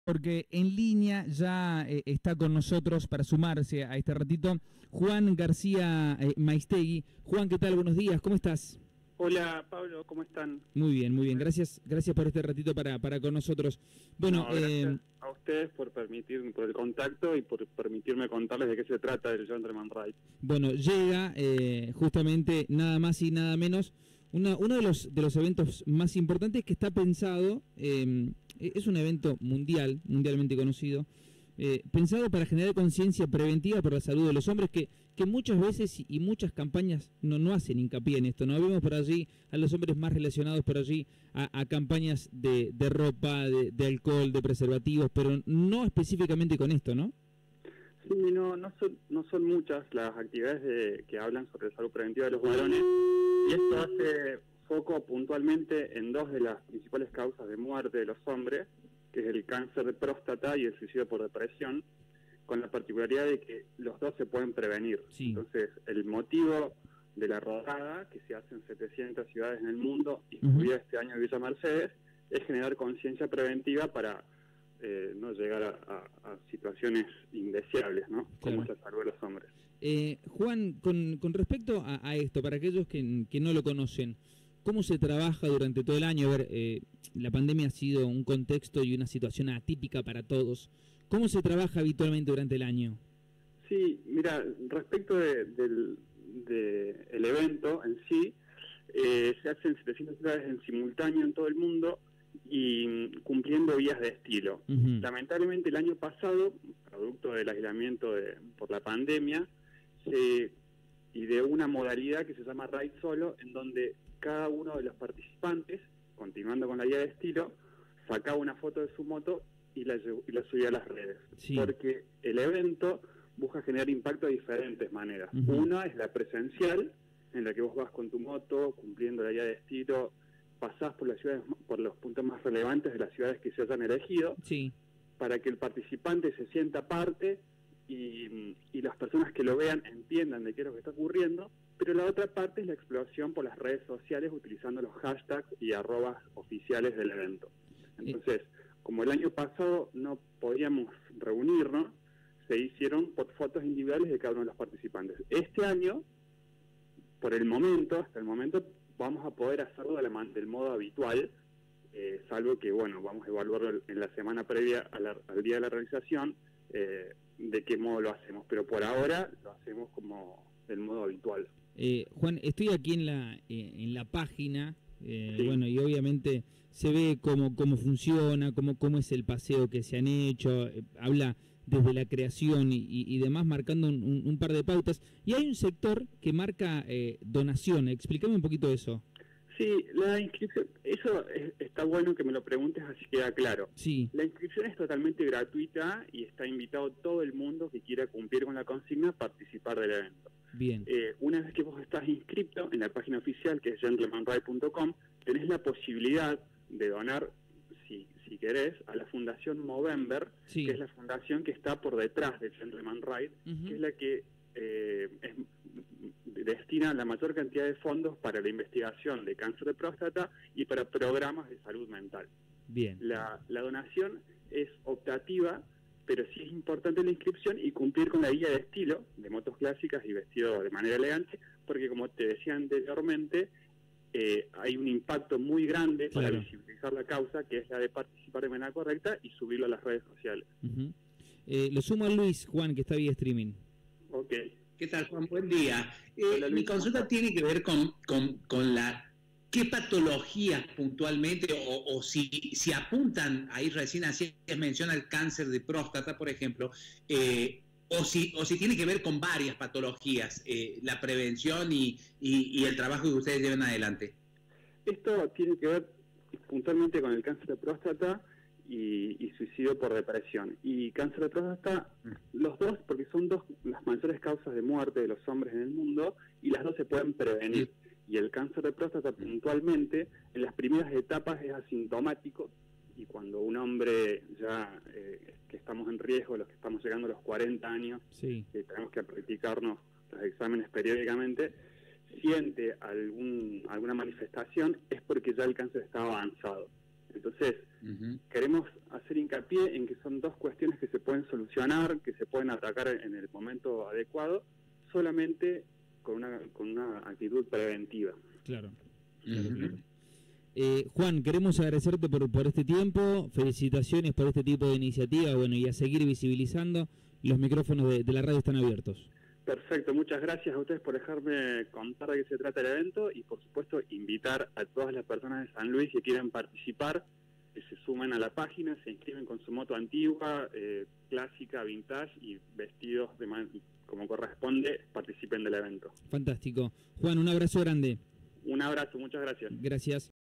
Juan García Maiztegui - Miembro de Comisión Organizadora
The Distinguished Gentleman’s Ride, es un evento mundial que tiene por desafío generar conciencia preventiva por la salud de los hombres.